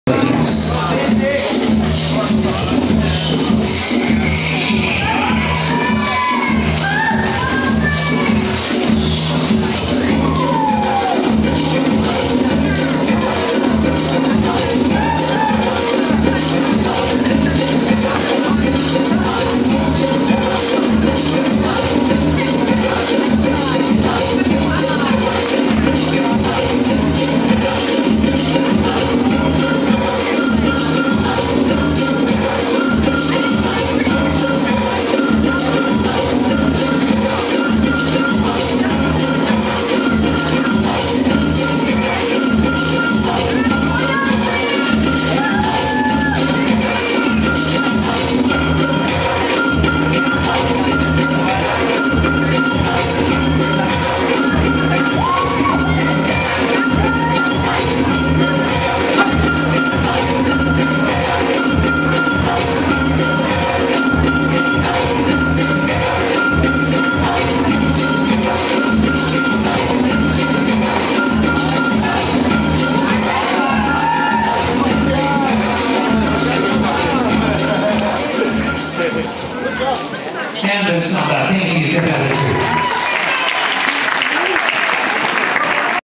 Stephen T. Nadeau & Natalia Skorikova dancing the Samba at the 2012 Paragon Open.